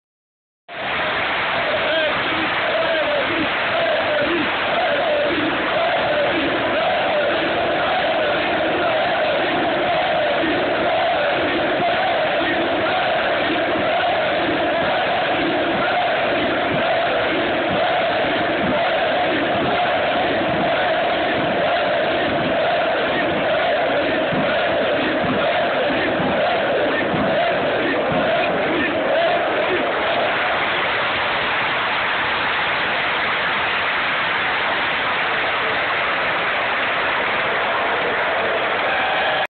Grobari - Vesely, Vesely, Vesely!!!
Partizan Belgrade fans (Grobari) paying homage to Jan Vesely at his last match for Partizan (vs Hemofarm Vrsac, June 8th, 2011)